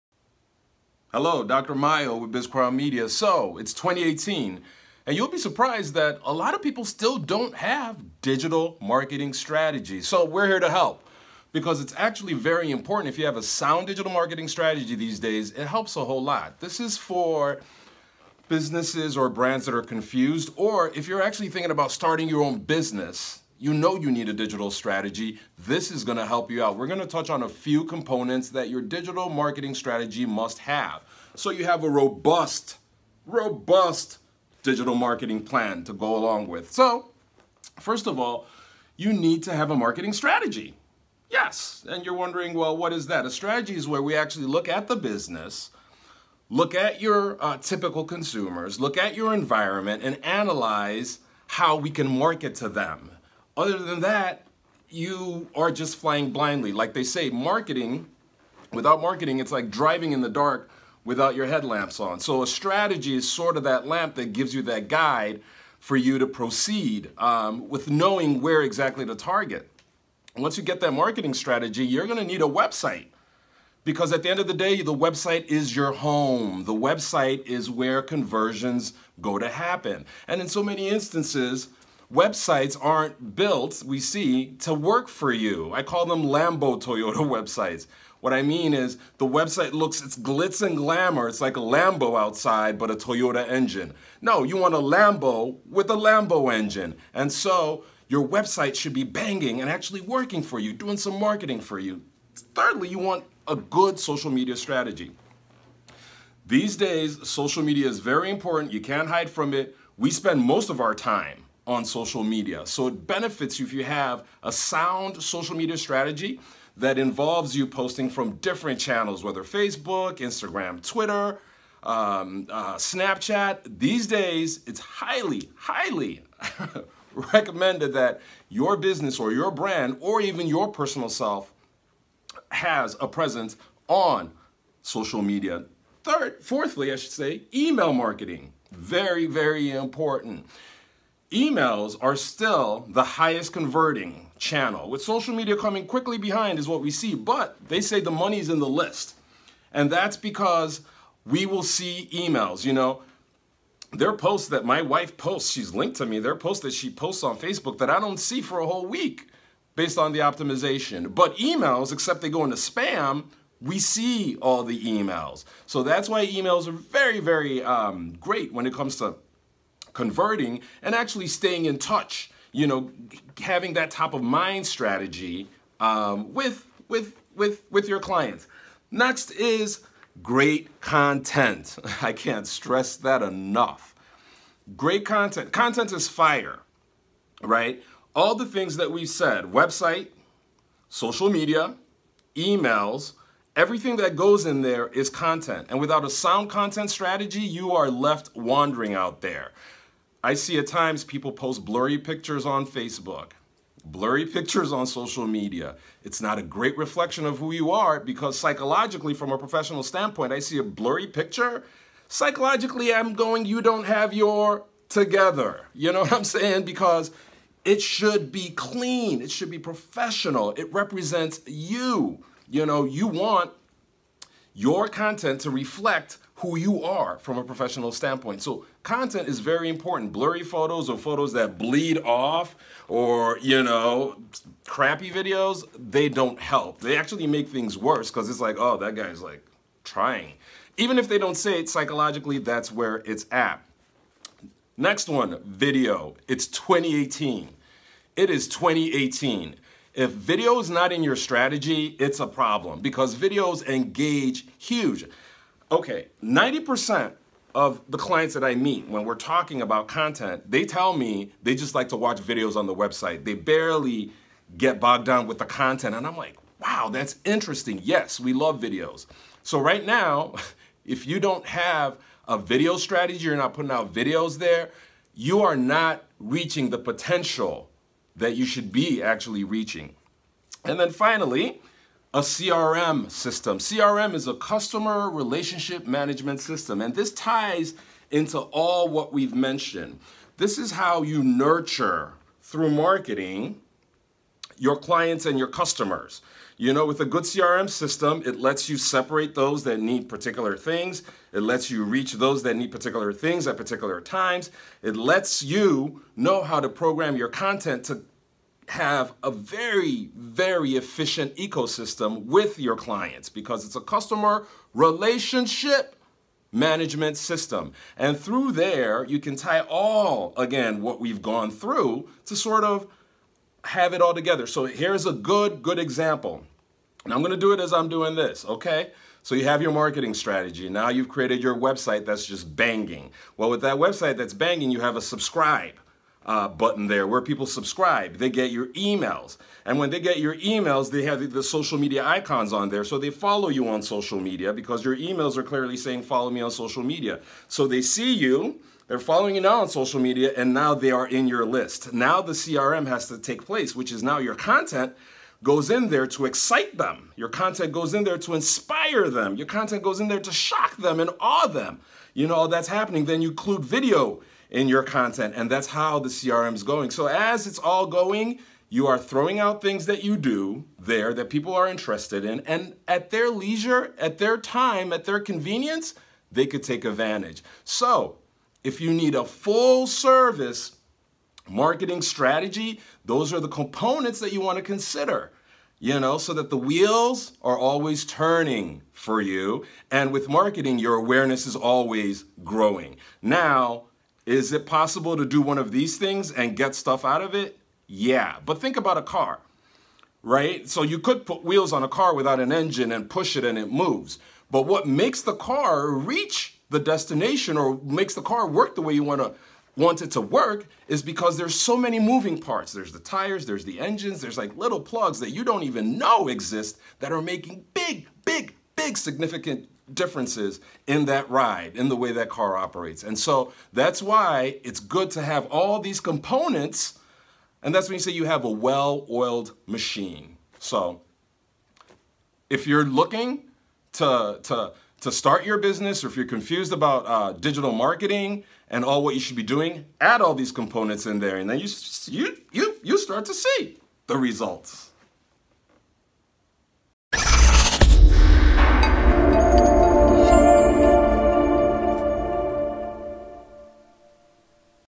Digital Marketing Ecosystem - Dr. Mayo - BizCrown Media
Digital marketing requires an ecosystem to be highly efficient. Think of all the components that make a car work, tires, doors, windows etc. without one of them, your ride wont be as comfortable... the same goes for digital marketing. Dr. Mayo Adegbuyi breaks down the components needed for an effective digital marketing ecosystem.